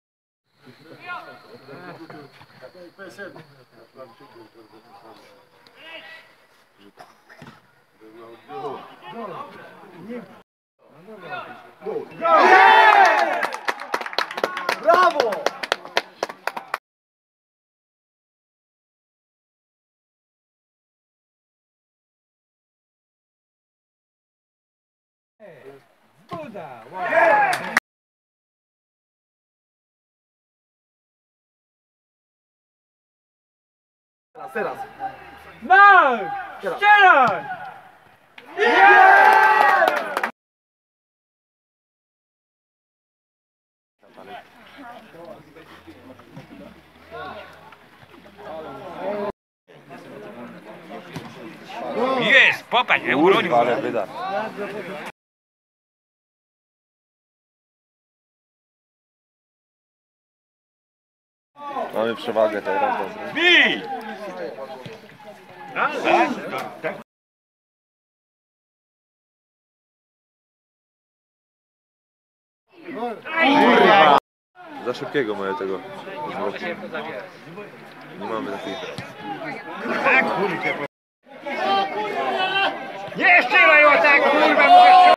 PP San Kłyżów OKS Mokrzyszów 5:1
Mecz 1/4 Pucharu Polski w Podokręgu Stalowa Wola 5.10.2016